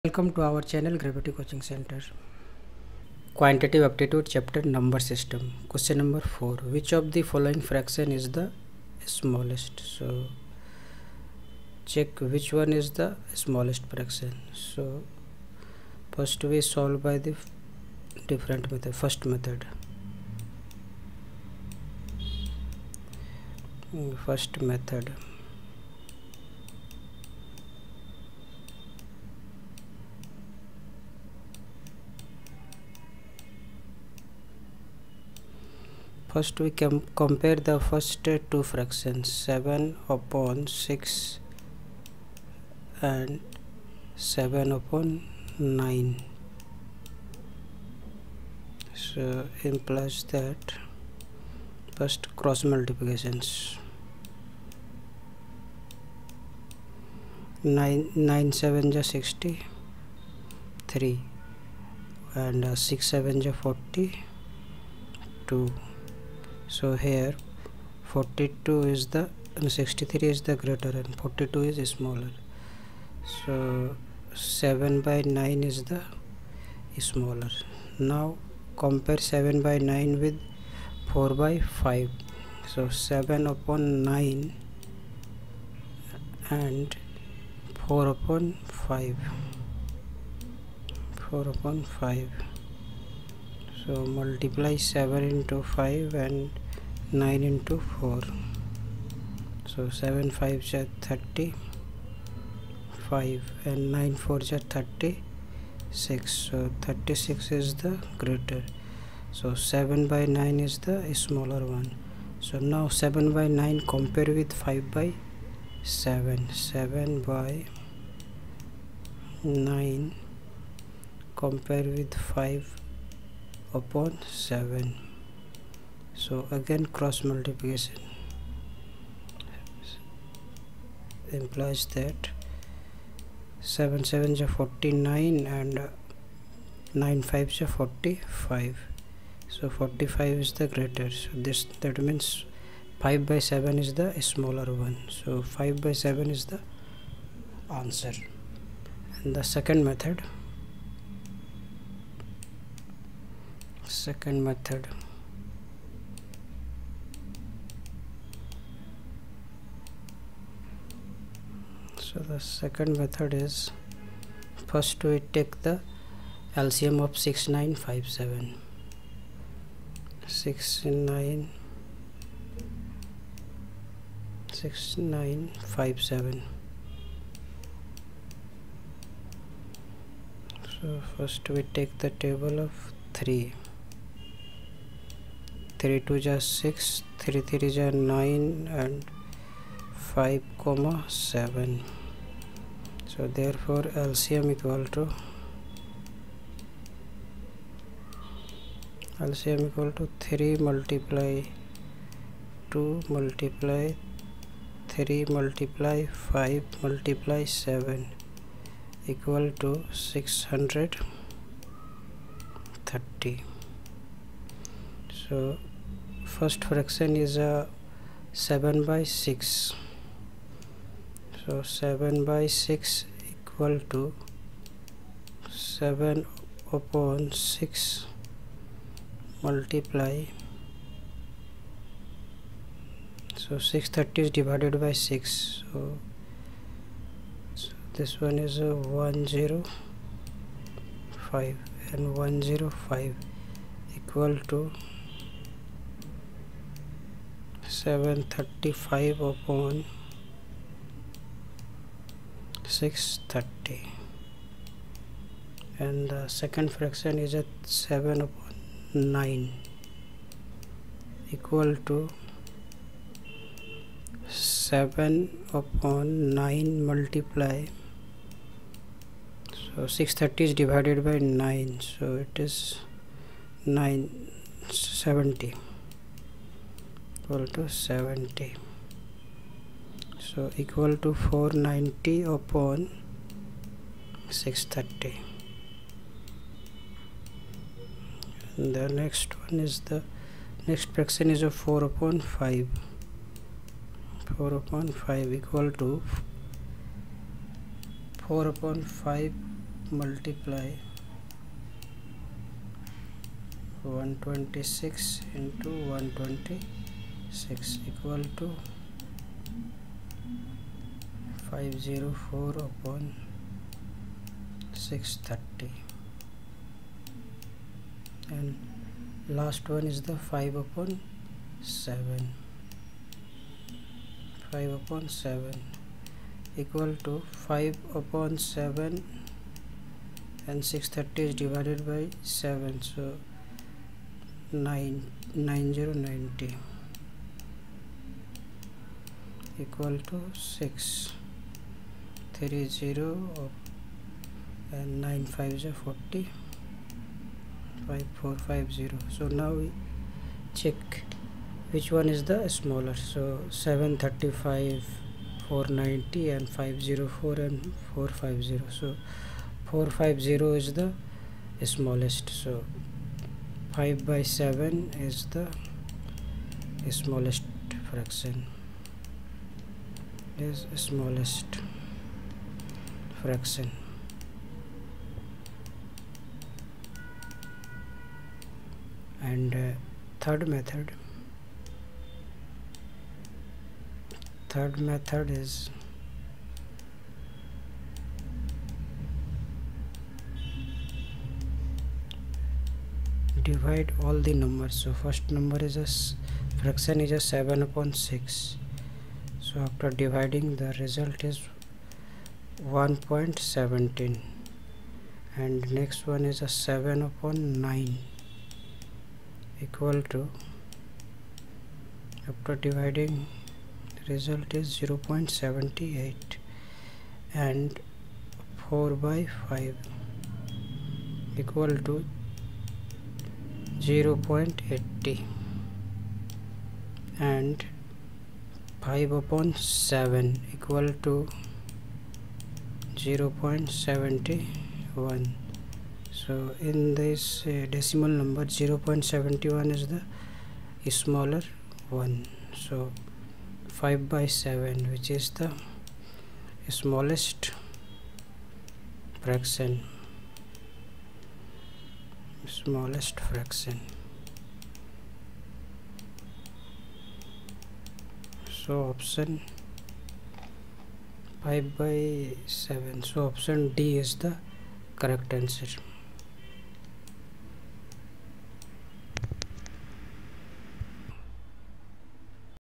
Q4 | Which of the following fraction is the smallest ? 7/6, 7/9, 4/5, 5/7 | Number System
Qu(4):- Which of the following fraction is the smallest ?